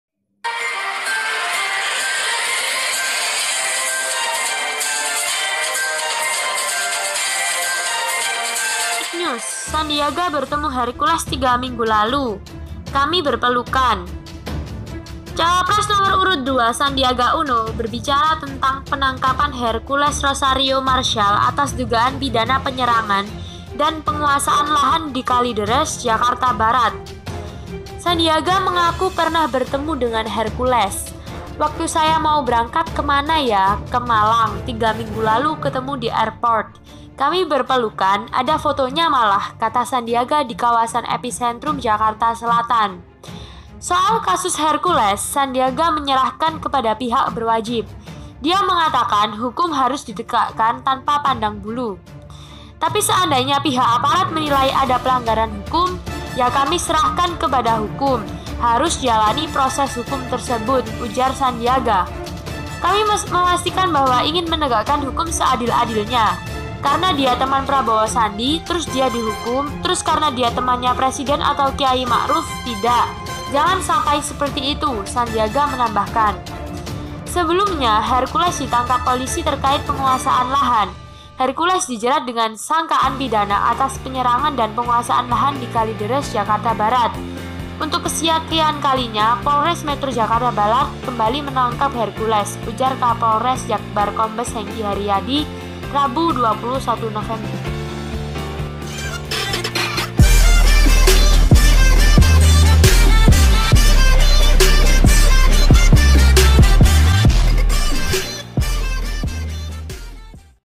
SANDIAGA PELUKAN DG HERCULES SBLM JADI TRSANGKA;KAWAN PRABOWO;PILPRES 2019;AIR PORT;JOKOWI;MA'RUF A
Jakarta - Cawapres nomor urut 02 Sandiaga Uno berbicara tentang penangkapan Hercules Rosario Marshal atas dugaan pidana penyerangan dan penguasaan lahan di Kalideres, Jakarta Barat. Sandiaga mengaku pernah bertemu dengan Hercules.
"Waktu saya mau berangkat ke mana ya, ke Malang, 3 minggu lalu ketemu di airport. Kami berpelukan, ada fotonya malah," kata Sandiaga di kawasan Epicentrum, Jakarta Selatan, Kamis (22/11/2018).
Baca juga: Hercules, dari P
Soal kasus Hercules, Sandiaga menyerahkan kepada pihak berwajib. Dia mengatakan hukum harus ditegakkan tanpa pandang bulu.
"Tapi seandainya pihak aparat menilai ada pelanggar, ya kami serahkan kepada hukum, harus jalani proses hukum tersebut," ujar Sandiaga.
"Kami memastikan bahwa ingin menegakkan hukum seadil-adilnya. Karena dia teman Prabowo-Sandi, terus dia dihukum, terus karena dia temannya Presiden atau Kiai Ma'ruf tidak, jangan sampai seperti itu," Sandiaga menambahkan.
Baca juga: Hercules Ditap Lagi
Sebelumnya, Hercules dit terkait penguasaan lahan. Hercules dijerat dengan sangkaan pidana atas penan dan penguasaan lahan di Kalideres, Jakarta Barat.
"Untuk kesekian kalinya, Polres Metro Jakarta Barat kembali menangkap Heres," ujar Kapolres Jakbar Kombes Hengki Haryadi, Rabu (21/11).